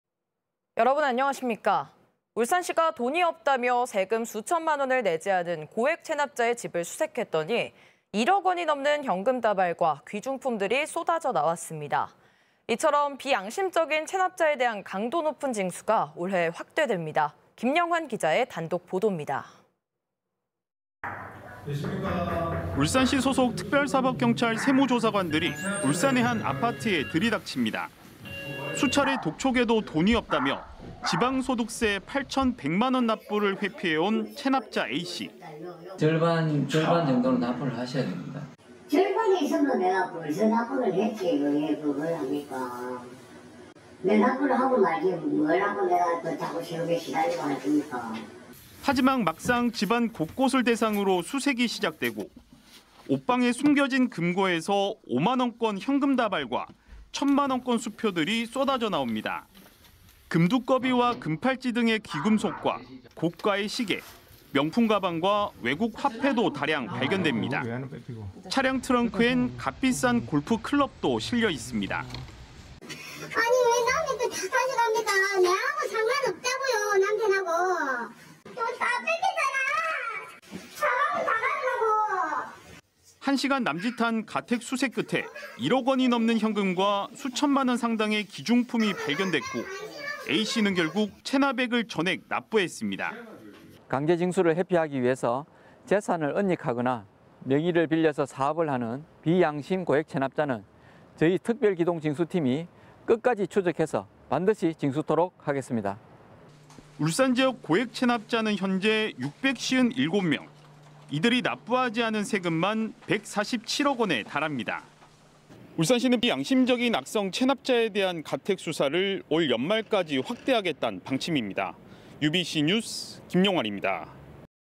악성체납자 집에서 현금 다발 '와르르' 240517
(앵커멘트)

울산시가 돈이 없다며
세금 수천만 원을 내지 않은
고액 체납자의 집을 수색했더니
1억 원이 넘는 현금 다발과
귀중품들이 쏟아져 나왔습니다.

이처럼 비양심적인
체납자에 대한 강도 높은
징수가 올해 확대됩니다.

김영환 기자의 단독 보돕니다.

(리포트)
울산시 소속
특별사법경찰 세무조사관들이
울산의 한 아파트에
들이닥칩니다.

수차례 독촉에도 돈이 없다며
지방소득세 8천100만 원
납부를 회피해 온 체납자 A씨.

(싱크) '절반 정도는 납부를 하셔야 됩니다.'/ '절반 있으면 내가 벌써 납부를 했지 이렇게 합니까? 내가 납부를 하고 말지 뭐 하려고 내가 자꾸 세금에 시달리겠습니까?'

하지만 막상 집안 곳곳을
대상으로 수색이 시작되고,

옷방에 숨겨진 금고에서
5만 원권 현금 다발과
천만 원권 수표들이
쏟아져 나옵니다.

금두꺼비와 금팔찌 등의
귀금속과 고가의 시계,
명품 가방과 외국 화폐도
다량 발견됩니다.

차량 트렁크엔 값비싼
골프클럽도 실려있습니다.

(싱크) '아니 왜 남의 것을 다 가져갑니까. 나하고 상관없다고요. 남편 하고.. 돈 다 빼앗겼잖아. 가방도 다 가져가고..'

1시간 남짓한 가택 수색 끝에
1억 원이 넘는 현금과
수천만 원 상당의 귀중품이
발견됐고, A씨는 결국
체납액을 전액 납부했습니다.

(인터뷰)조해진/울산시 특별기동징수팀장 '강제징수를 회피하기 위해서 재산을 은닉하거나 명의를 빌려서 사업을 하는 비양심 고액체납자는 저희 특별기동징수팀이 끝까지 추적해서 반드시 징수토록 하겠습니다.'

울산지역 고액체납자는
현재 657명.

이들이 납부하지 않은 세금만
147억 원에 달합니다.

울산시는
비양심적인 악성체납자에 대한
가택수사를 올 연말까지
확대하겠단 방침입니다.

ubc뉴스 김영환입니다.